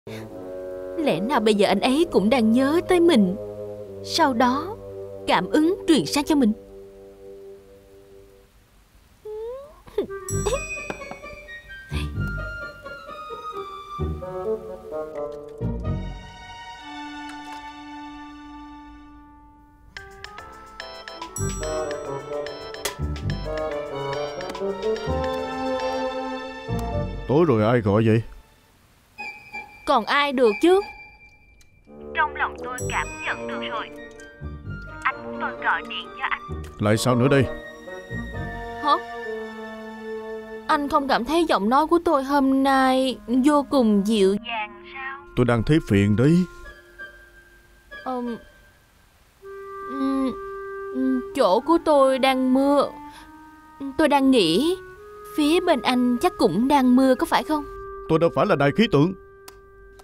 Đang Nhớ Gọi Điện Hỏi Thăm Tí Mà Bị C/h/ử/i Không Thương Tiếc
Đang Nhớ Gọi Điện Hỏi Thăm Tí Mà Bị Chửi Không Thương Tiếc

Âm nhạc